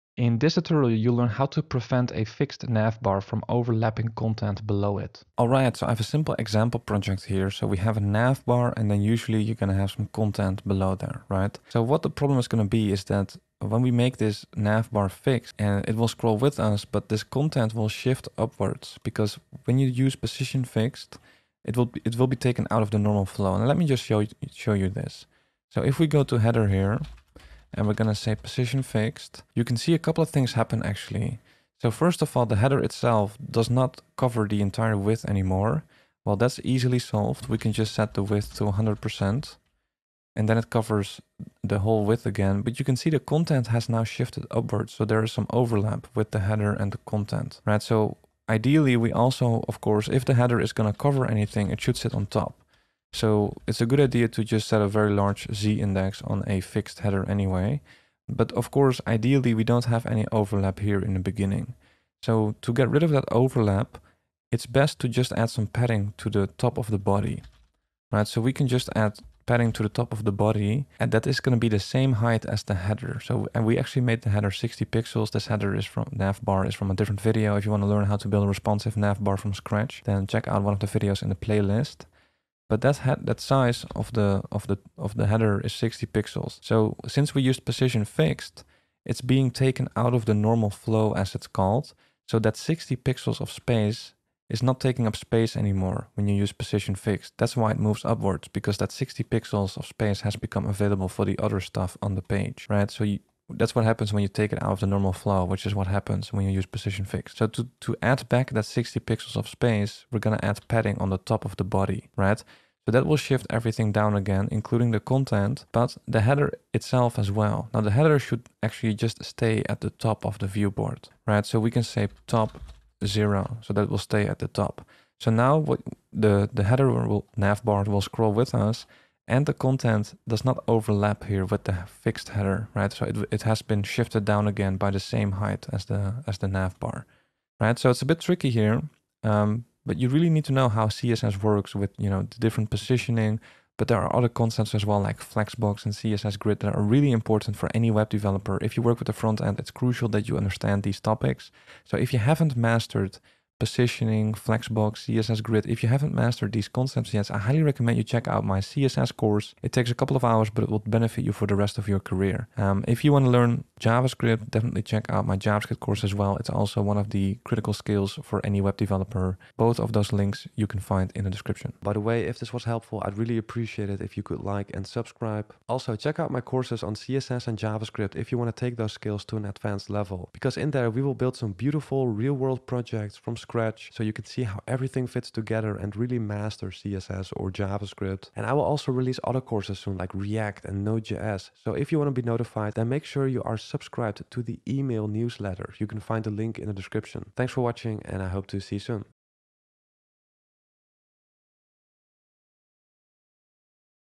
Prevent Fixed Navbar from Overlapping Content (Content Shifting Upwards)
Hi, I'm Wesley. I'm a brand ambassador for Kinde (paid sponsorship).

0:00 Solution